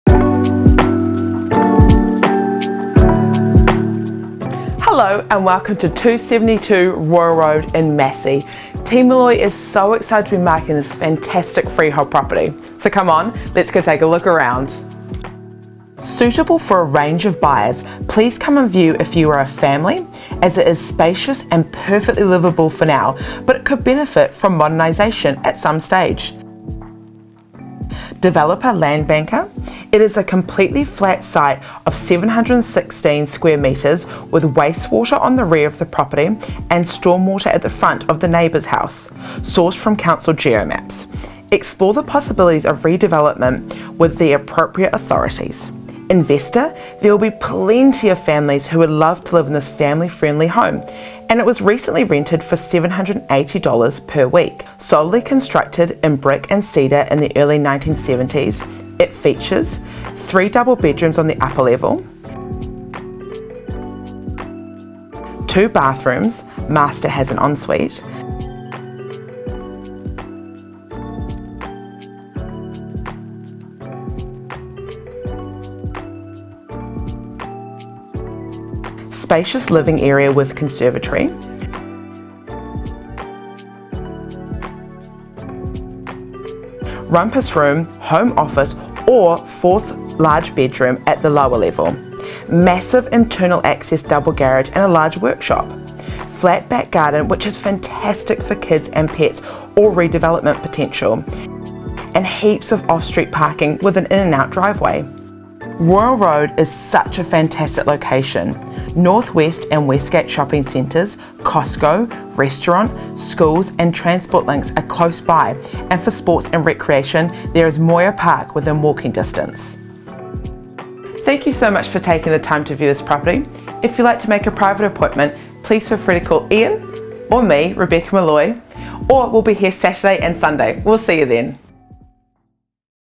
SOLD - 272 Royal Road, Massey - Ian Molloy and Lisa Molloy
First Time For Sale in 34 Years

Suitable for a range of buyers, please come and view if you are a
• Family. It is spacious and perfectly liveable for now but could benefit from modernisation at some stage.
• Developer/Land ...

Property Marketed by: Ian Molloy, Glenfield and Lisa Molloy, Glenfield